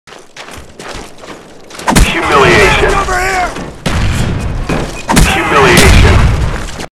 Double humilation on my mate and brother.
Taking out my mate (C0mm4nd3r C00L) and my brother (KrissJ87) with a double humiliation, much loli'ng resulted from this.